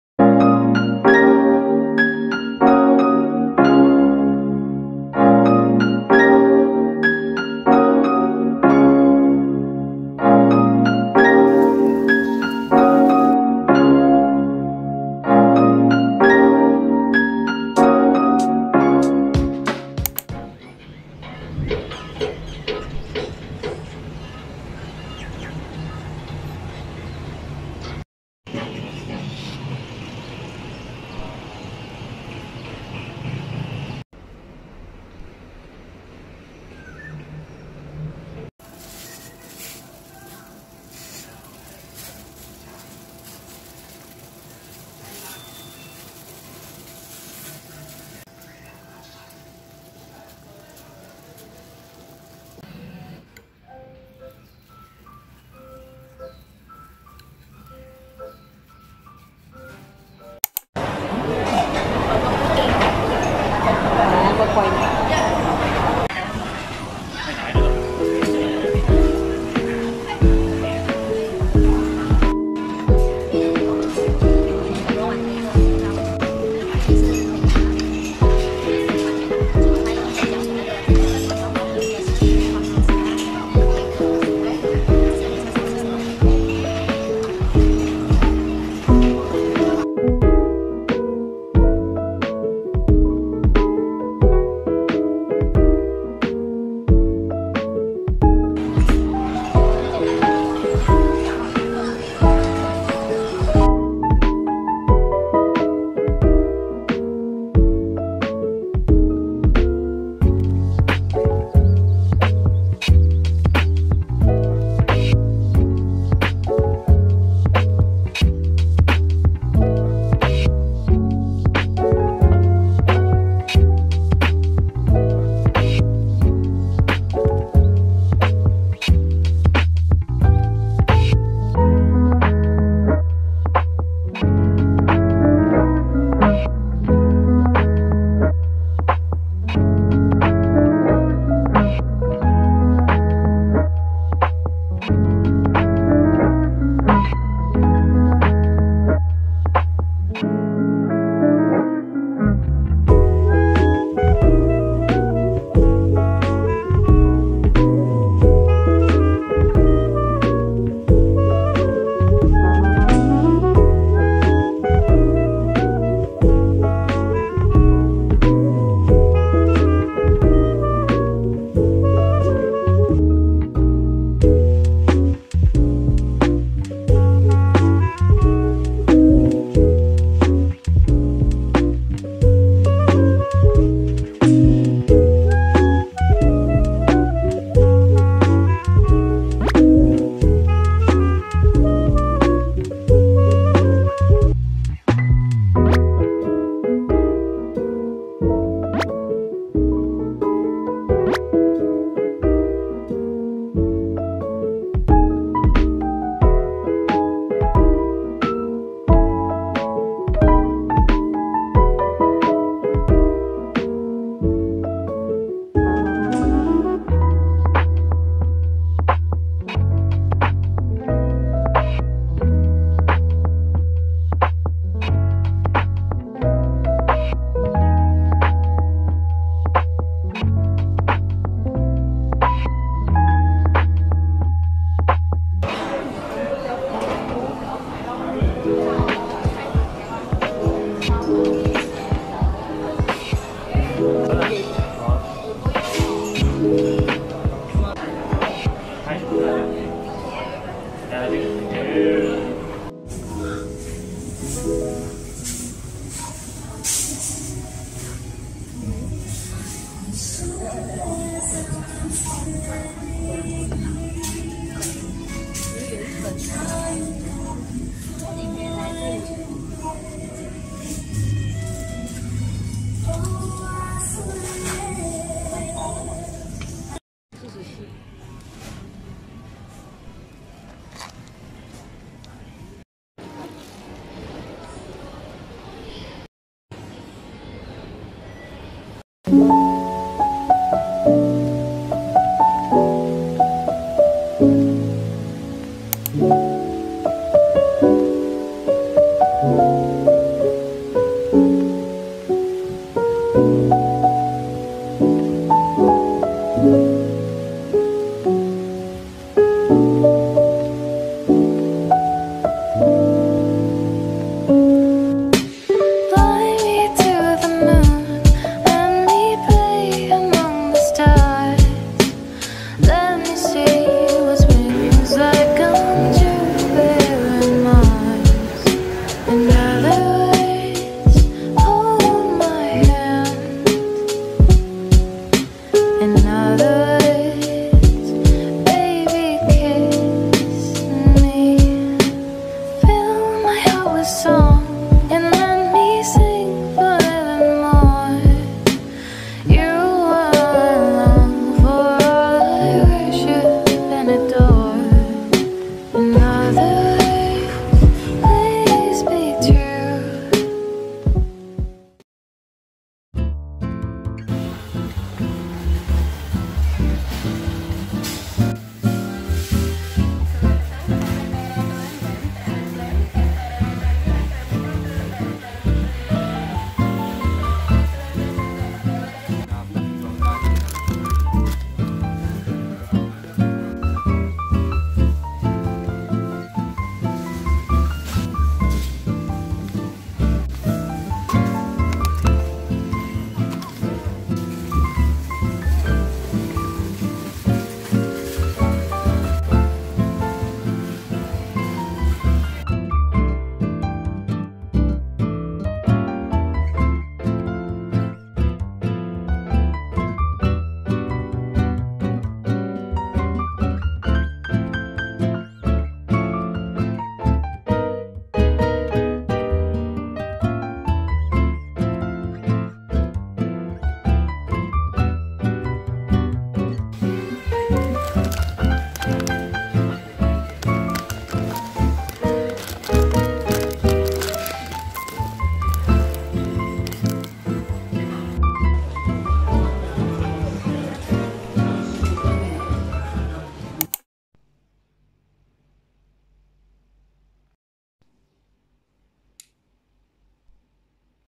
A Day In My Life Vlog| Haidilao Hotpot, Grocery Shopping, Penang Hawker Foods| Malaysia🌞
2) Please watch the video in 1080p HD :)

Ingredients I added to make the Chinese Healthy Tea:
1. Red dates (12pcs)
2. Goji Berries (one handful)
3. Dried Longan (one handful)
4. Ginger (3 small slices)
5. Sweetened Melon (4 small slices)
*Rinse all the ingredients with water and place all into a pot and add in plain water
*Boil it until cook, then you are ready to enjoy :)

Thank you for clicking in this video, hope you enjoy watching it. See you in my next video, bye :)